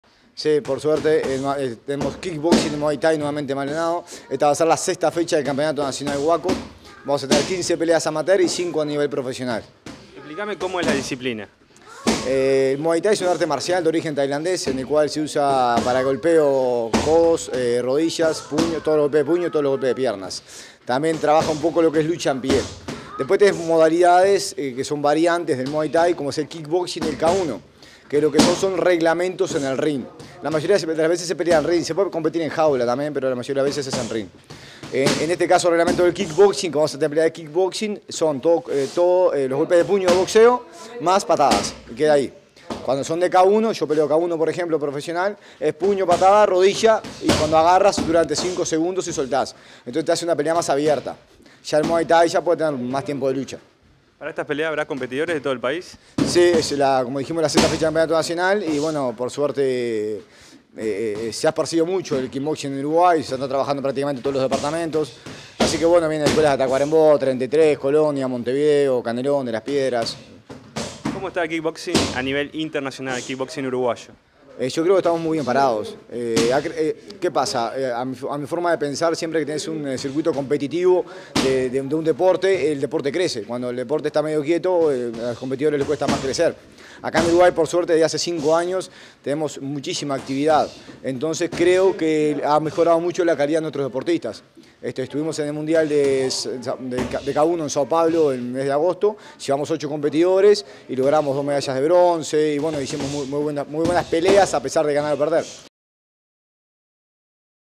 CAMPEONATO NACIONAL WAKO - 8 NOVIEMBRE EN MALDONADO
ENTREVISTA A LUIS PÉREZ, COMPETIDOR Y ORGANIZADOR.